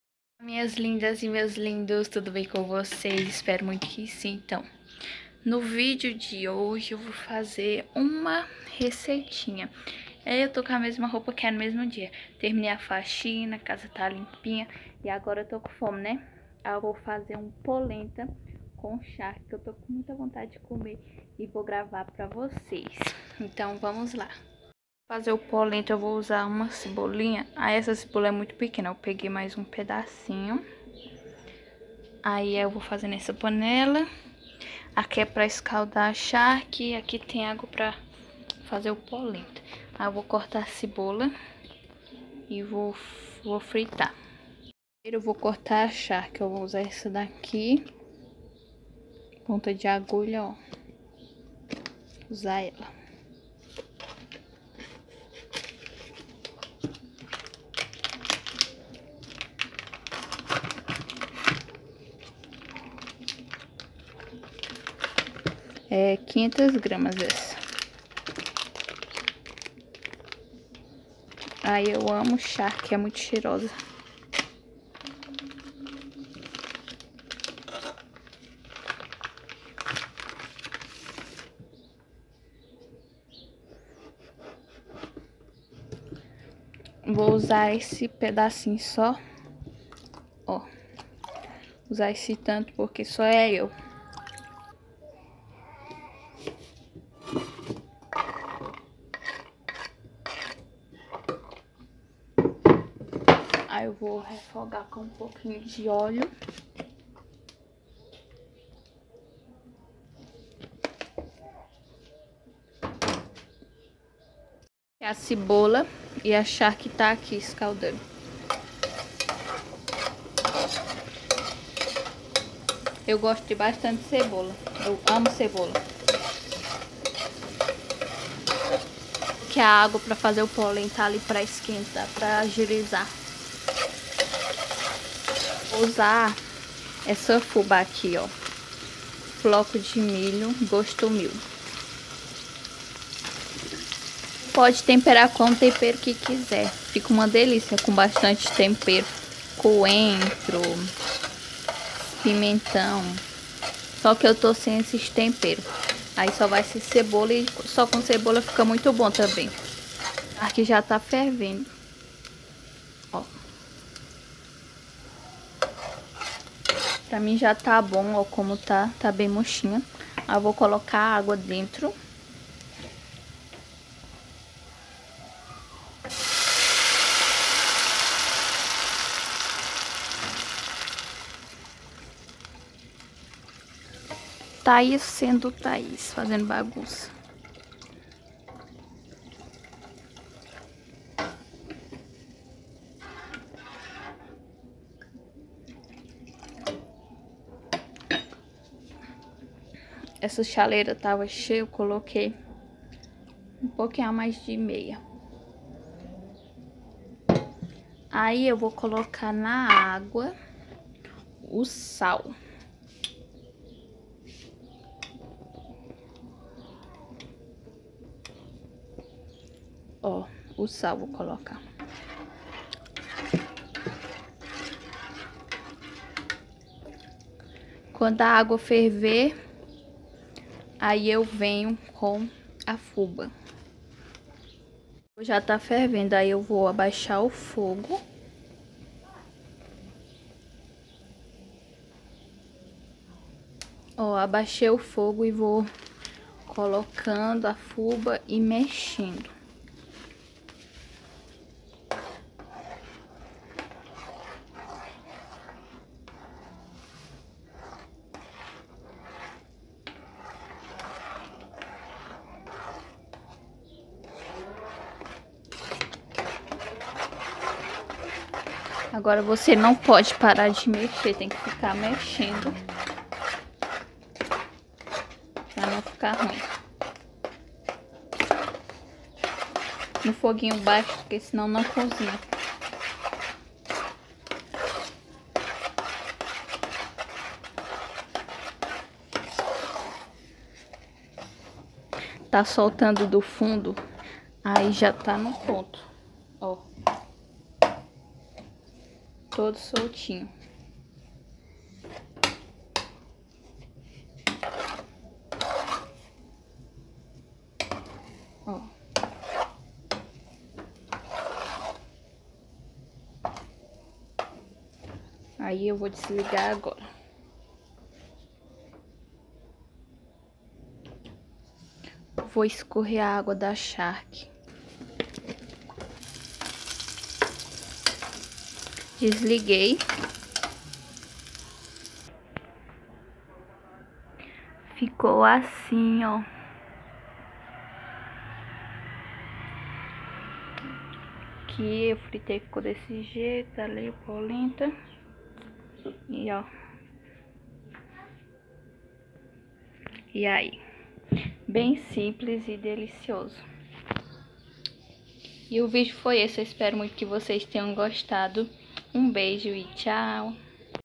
POLENTA COM CHARQUE (receita)
RECEITA MOLE :

4 xícaras de água
2 xícaras de fubá

RECEITA DURA :

3 xícaras de água
2 xícaras de fubá

Olá sou Taís Souza

tenho um Baby de 2 meses   chamado Rhavi